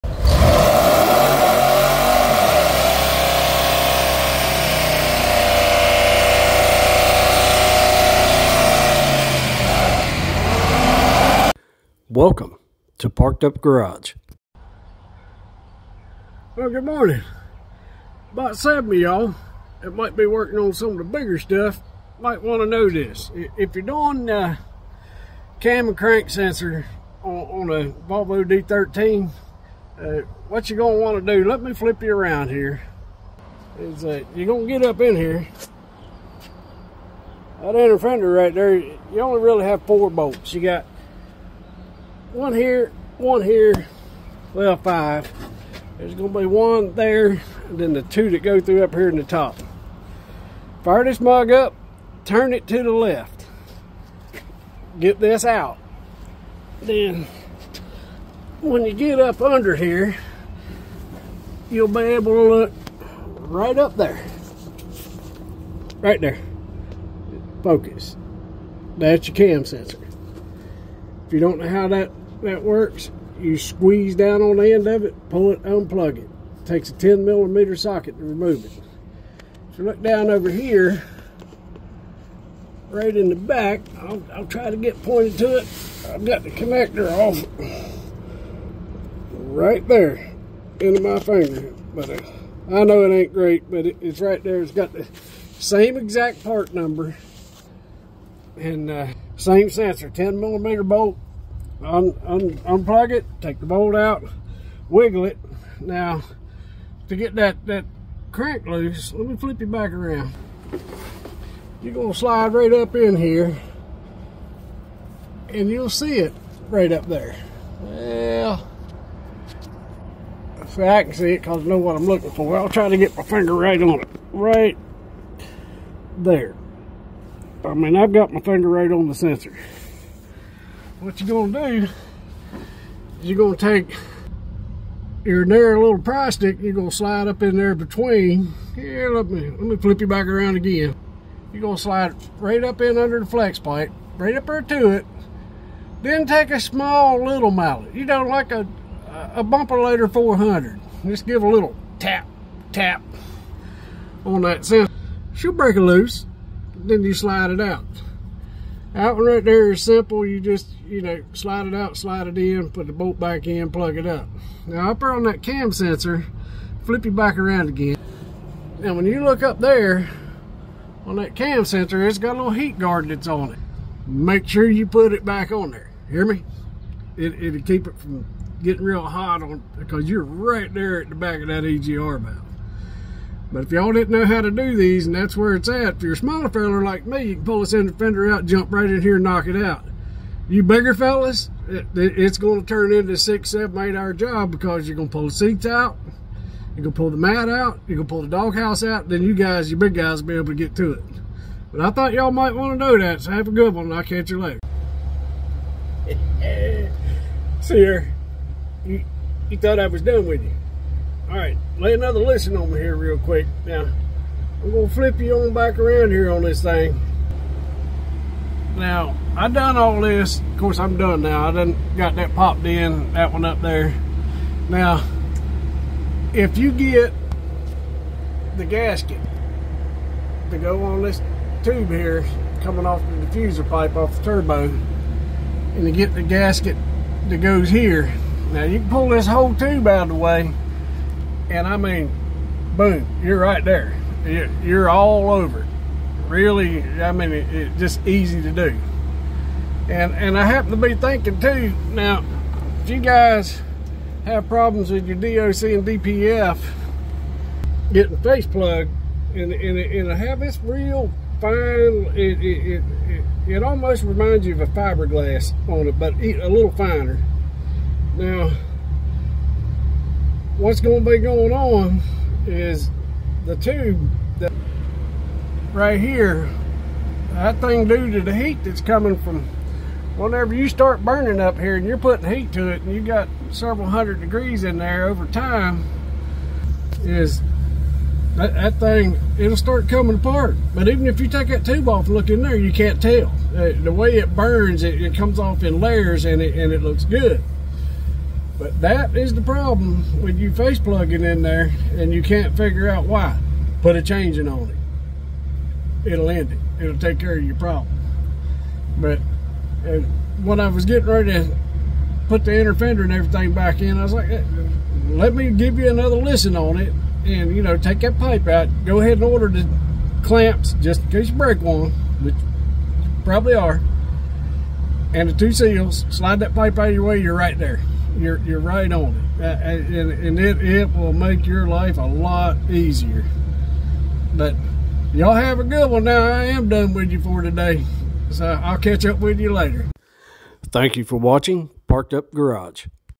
Volvo D13 cam and crank sensor installation
how to replace D13 cam and crank sensor